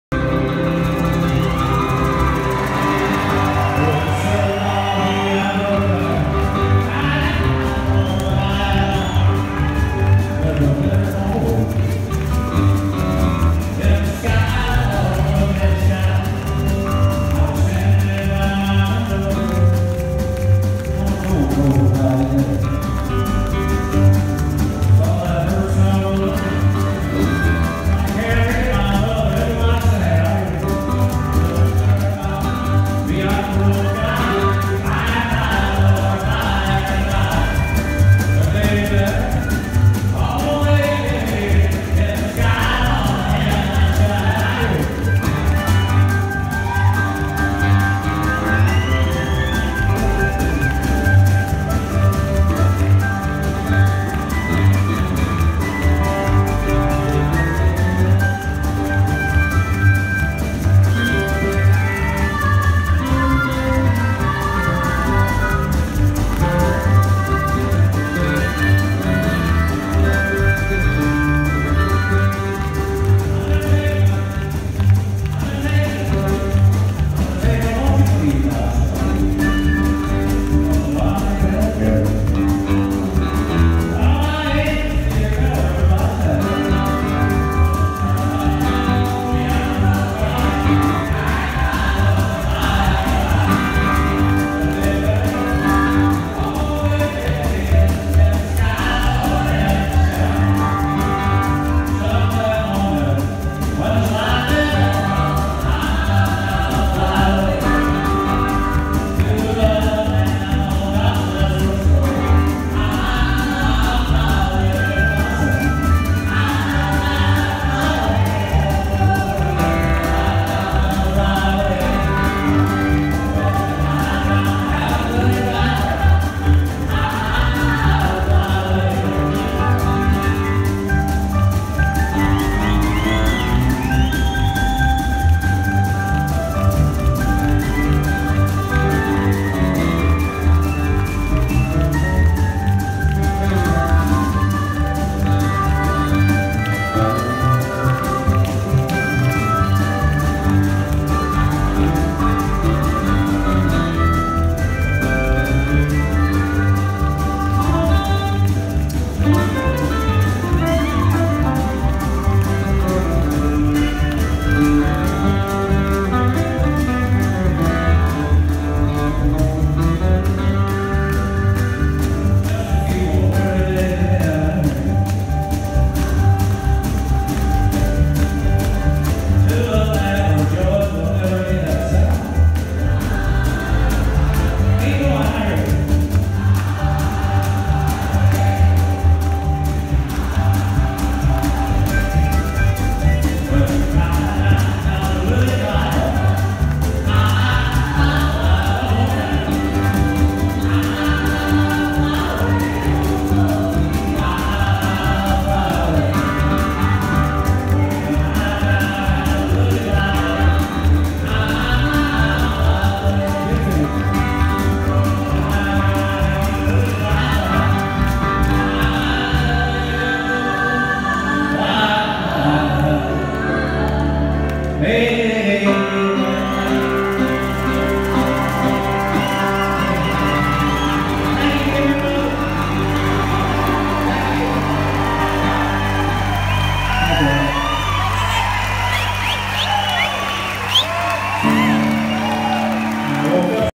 Jolie Goodnight with Willie Nelson & Kimmie Rhodes
Jolie Goodnight of Jolie & The Jackanapes joins Willie Nelson and Kimmie Rhodes at the Majestic Theatre in San Antonio, Texas.